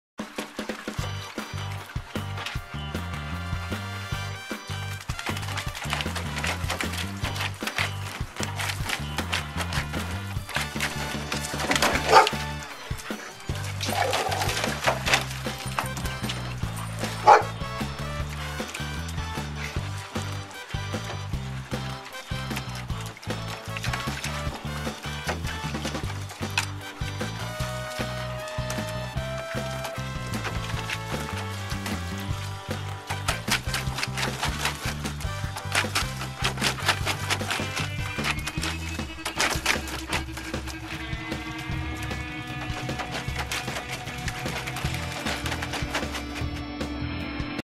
Pool Time... Again
It's officially summer at Columbus K9 when the pool comes out at Doggy Daycare!